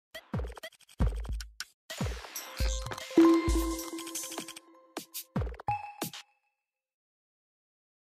Kanalımızın Yeni Jenerik Logosunu Yaptık
After Effect ile mrtkproductions için yenilenmesi gereken animasyonlu logosunu tamamlayıp yeniledik. Yeni haliyle artık karşınızda olacak hayırlı olsun...